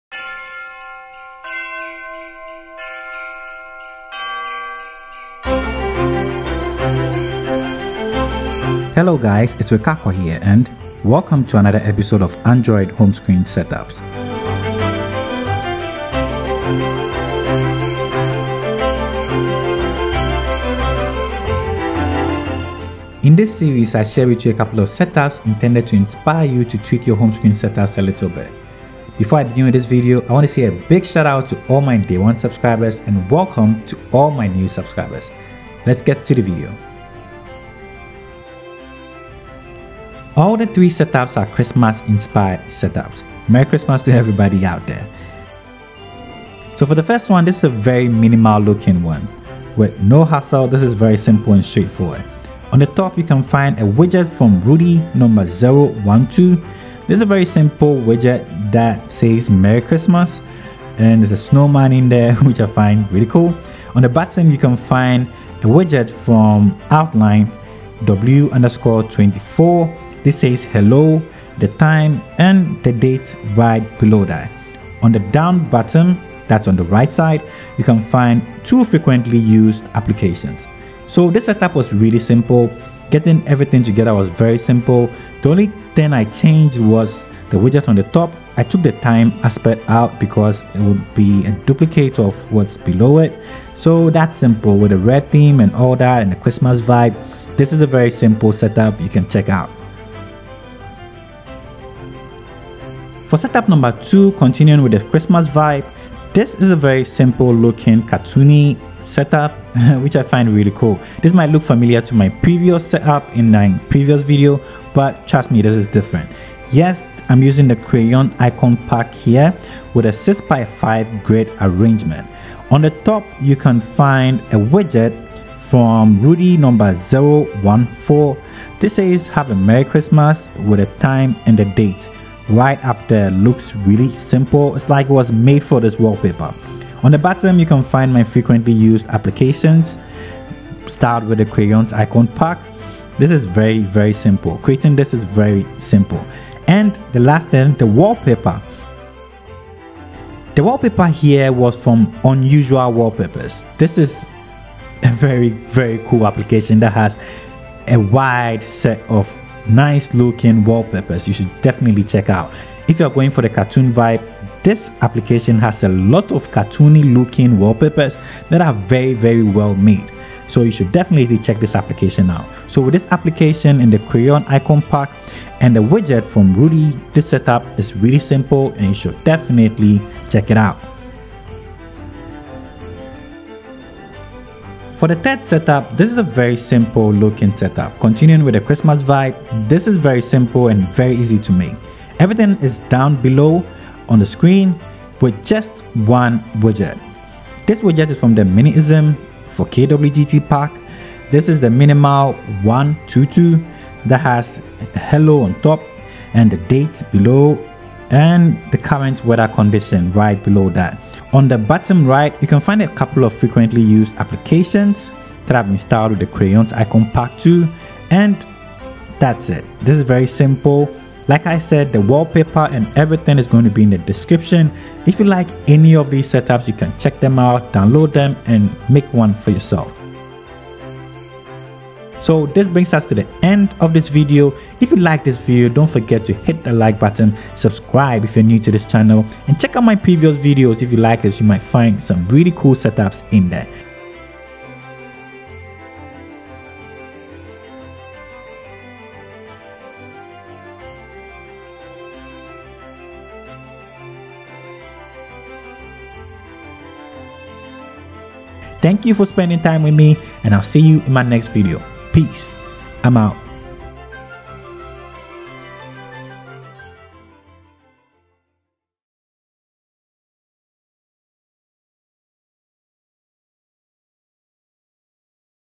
Best Android Homescreen Setups Of The Week Christmas edition ( Nova launcher & Lawnchair ))
Welcome to the christmas edition of ANDROID HOMESCREEN SETUPS

OUTLINE FOR KWGT
RUDIE FOR KWGT
O3 ICON PACK

CRAYONS ICON PACK
RUDIE FOR KWGT

MINI-ISM FOR KWGT